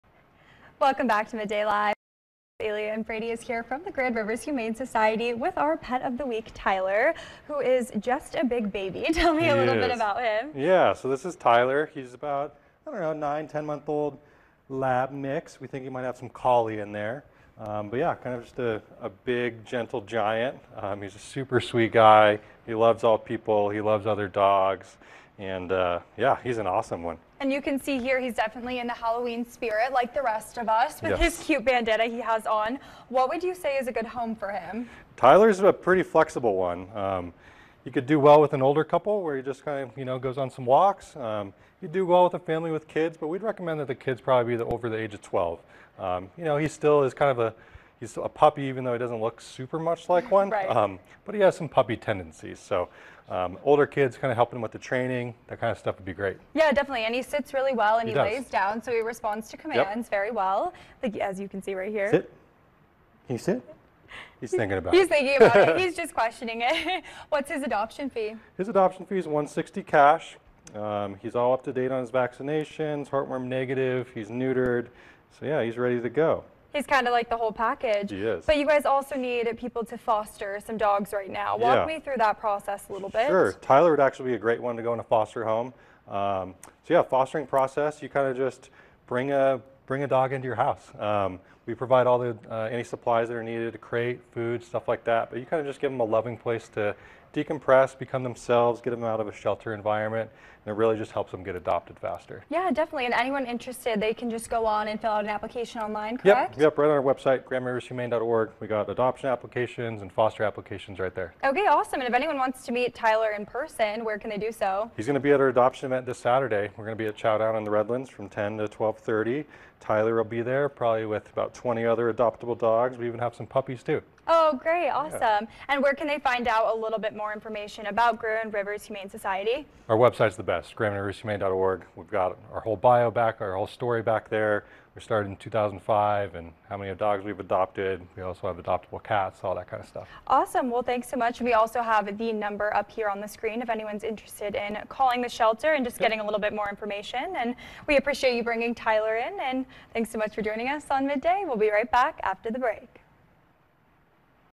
The Gentle Giant: Meet Tyler
Tyler is the Grand River Humane Society Pet of the Week